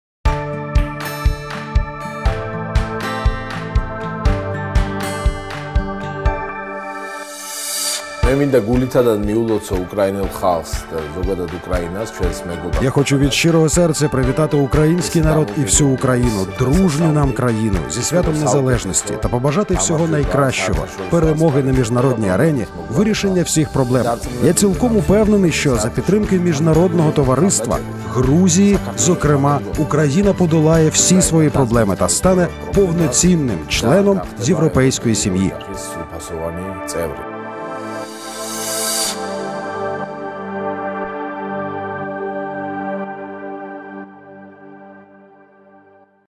Грузія
Посол Грузії в Україні Гела Думбадзе привітав нашу країну з Днем Незалежності!